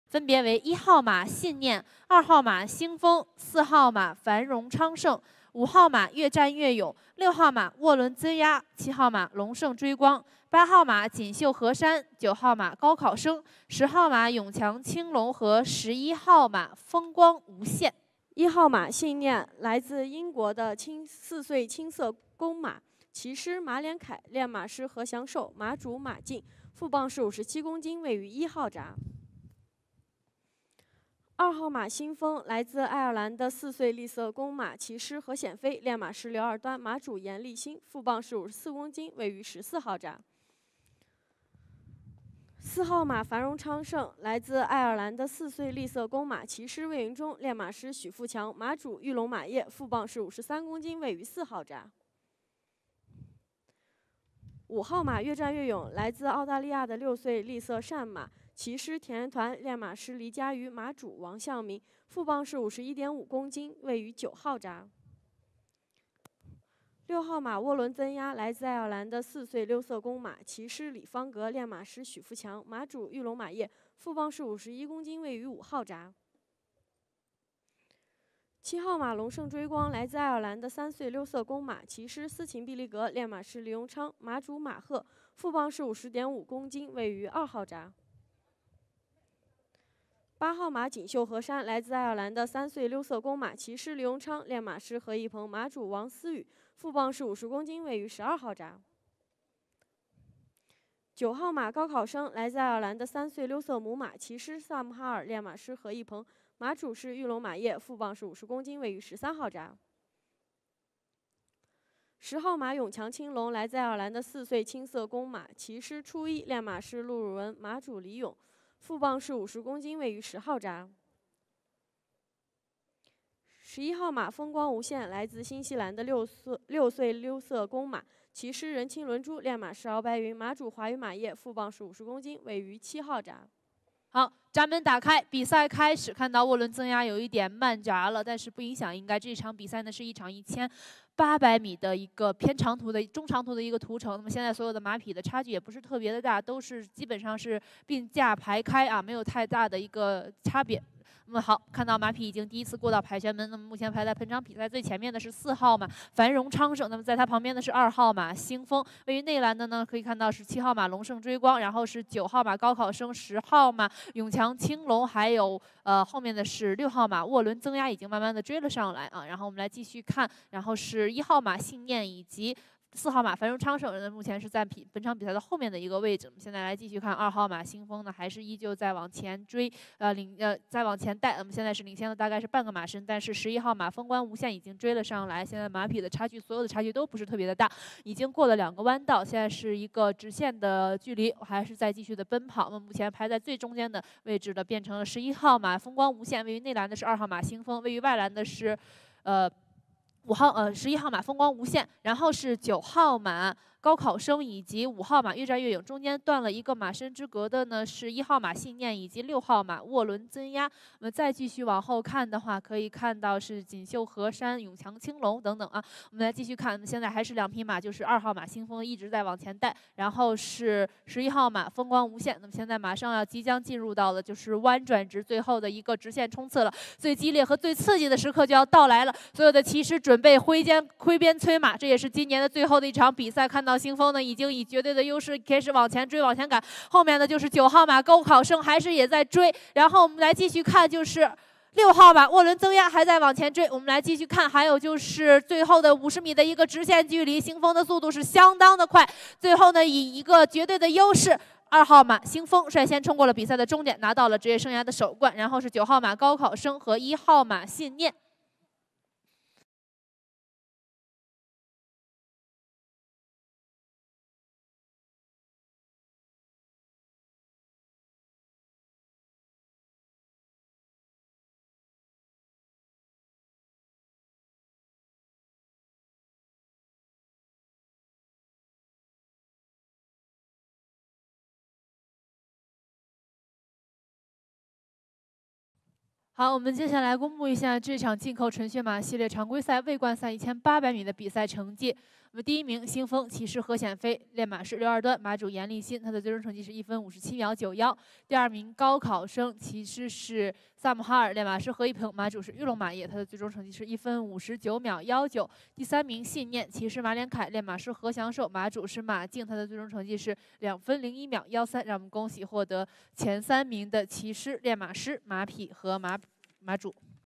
20230923 Imported Thoroughbred maiden 1800m (Race 9) 玉龙国际赛马公开赛D18 第9场 进口纯血马系列常规赛（未冠赛）1800米
Winner: Xing Feng (IRE) 星风, chestnut, colt, 4YO, by Galileo (IRE), from Stellar Wind (USA)

Commentator: Shan YAO